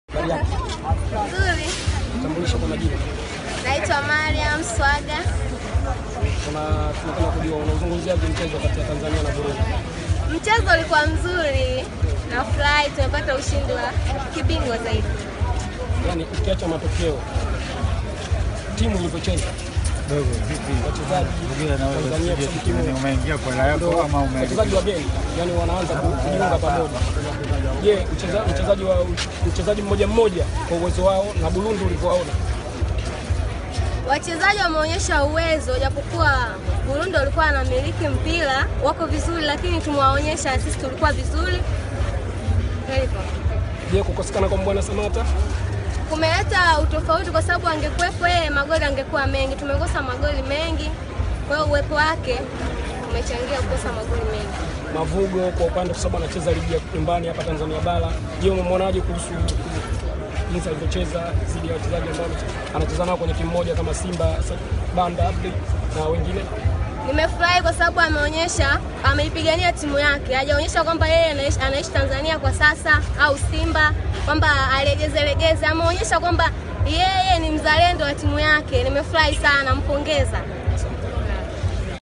Mavugo kaonesha uzalendo, Samatta angewatikisa Burundi
Shabiki wa Taifa Stars, Mariam Swaga akitoa maoni yake kuhusu Meggie ya Tanzania na Burundi iliyomalizika kwa ushindi wa mabao 2-1